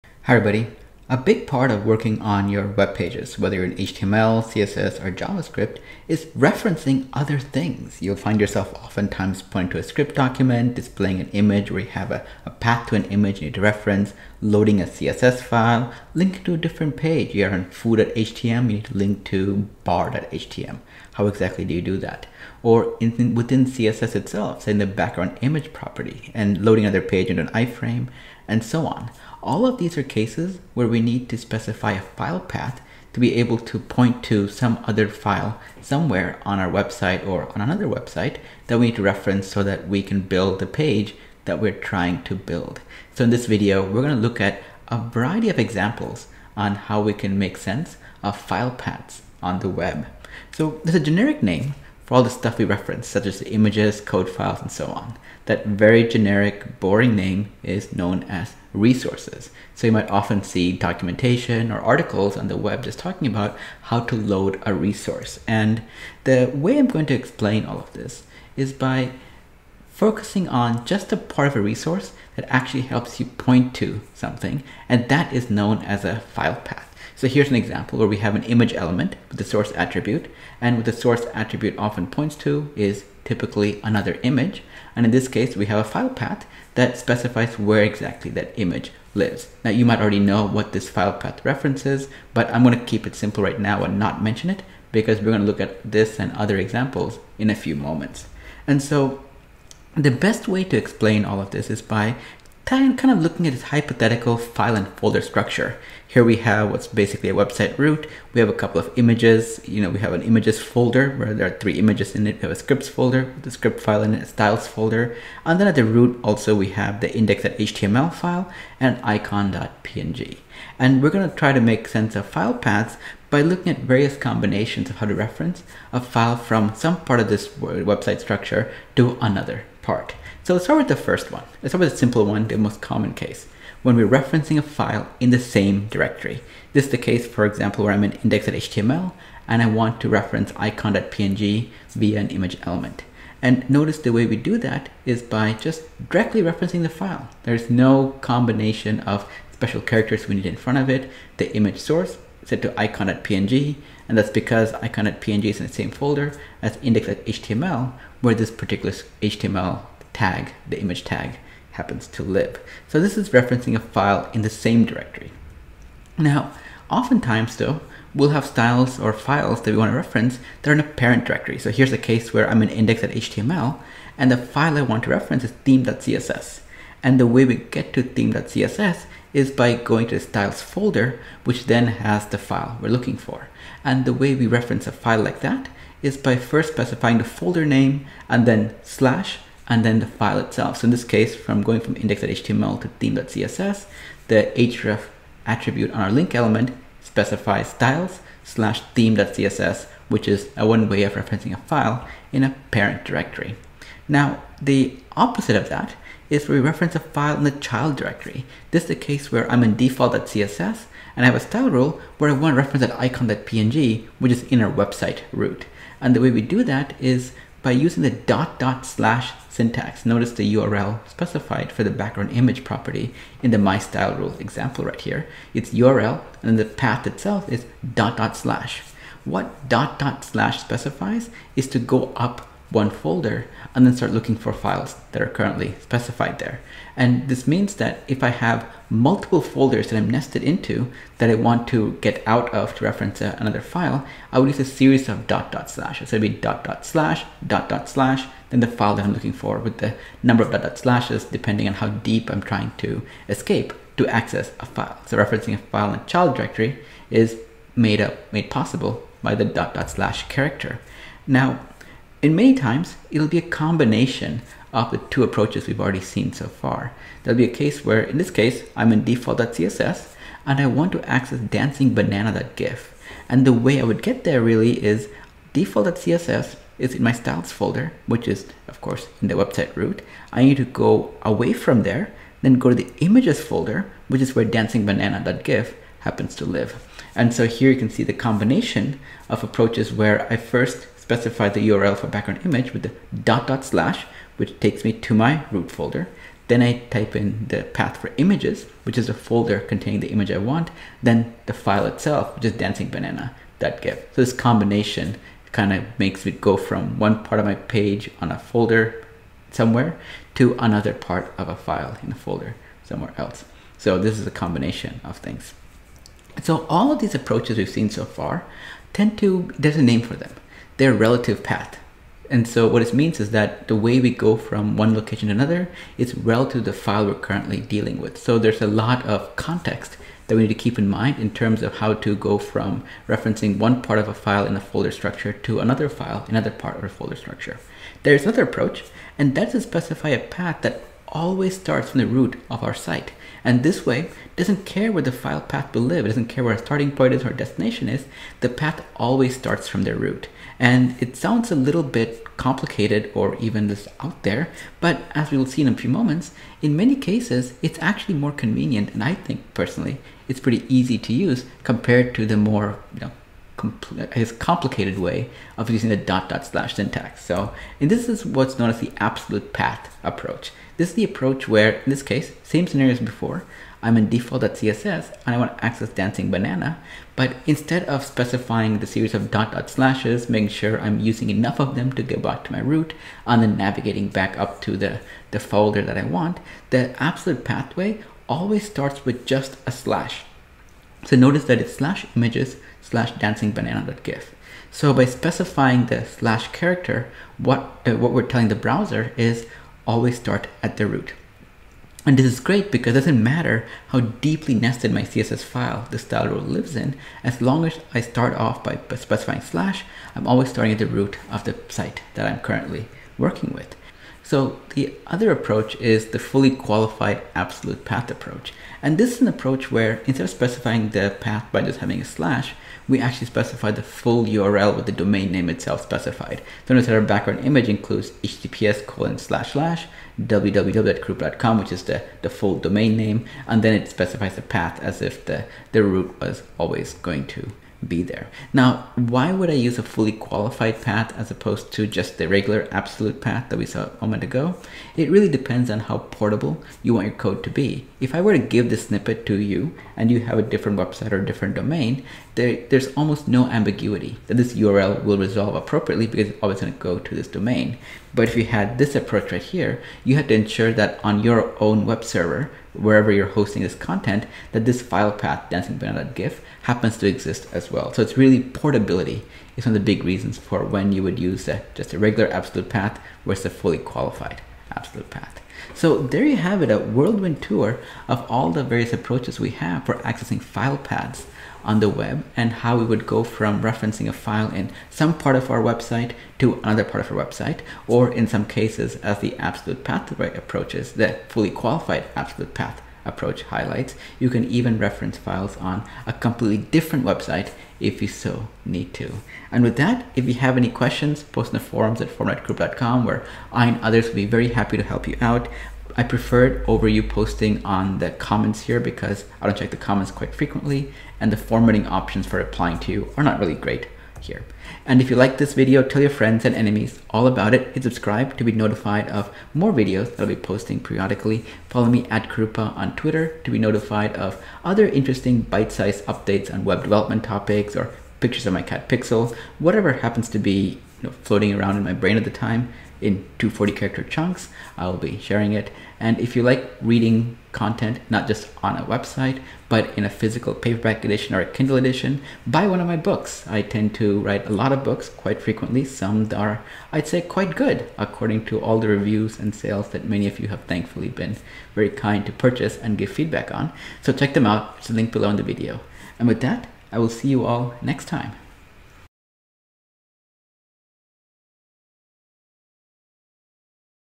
File Paths on the Web
Learn about the various ways you have for defining a file path to reference some resource in your web documents.